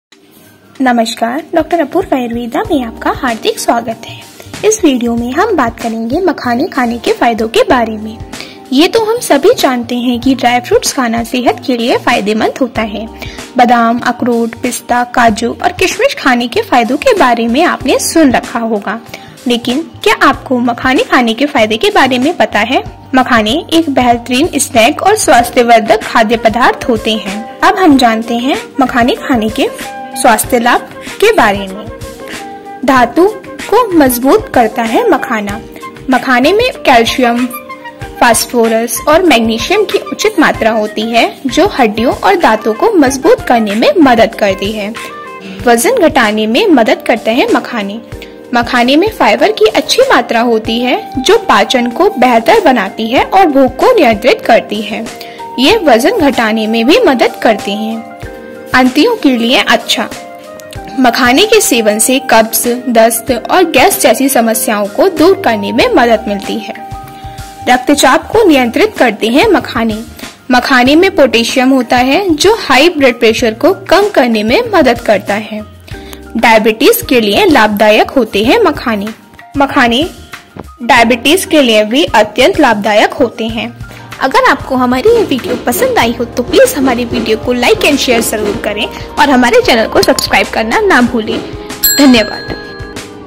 Makhana Health Benefits: सेहत का खज़ाना हैं मखाने, रोज़ खाएं - मखाना खाने के फायदे Fox Nuts Benefits
Makhana Health Benefits
Makhana, Lotus Seeds are also known as foxnuts. They are having super amazing health benefits. Strong Bones to weight loss to good digestion Makhana provides all. Find out more in this video about more health benefits of eating Makhana and the correct way of eating Makhanas.
मखाना एक ऐसी चीज़ है जो बच्चे बड़े सब के लिए सेहत मंद है और ढेरों सेहत के फ़ायदों से भरा हुआ है माखना | वज़न कम करने से लेकर मज़बूत हड्डियों तक सब देता है मखाना | जानिए मखाना खाने के फायदे और इस डाइट में शामिल करने का सही तरीका |

Welcome to Dr. Apurva Ayurveda.

lots of love ever and always ❤️

Dr. Apurva Ayurveda